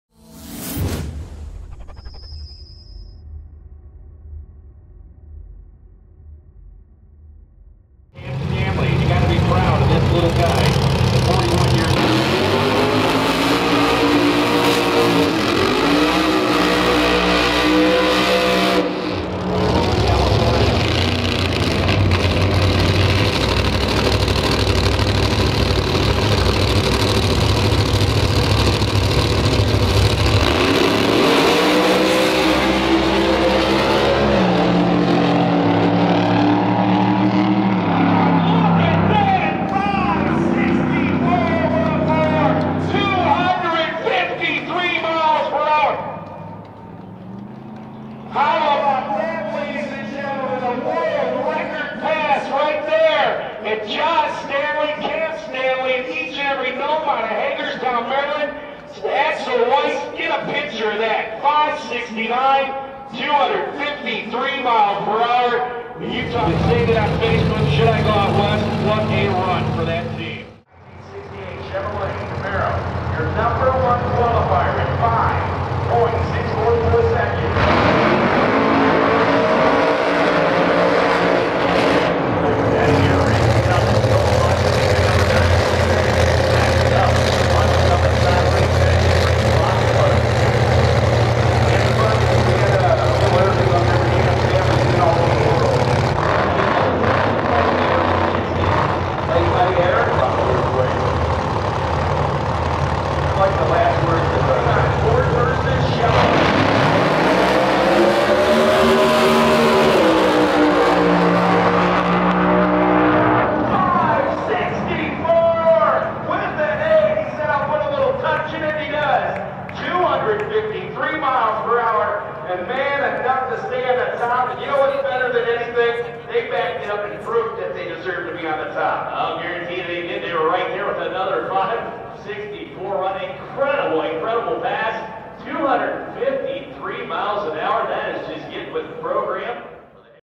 Stanley & Weiss 2013 Doorslammer record 5.64 @ 253
Las Vegas, Friday November 15, 2013. SCSN 9.Camp & John Stanley set a new world Record of 5.644 @ 253.04 in first round qualifying and backed it up.with a 5.648 in the second round qualifying to achieve the new doorslammer record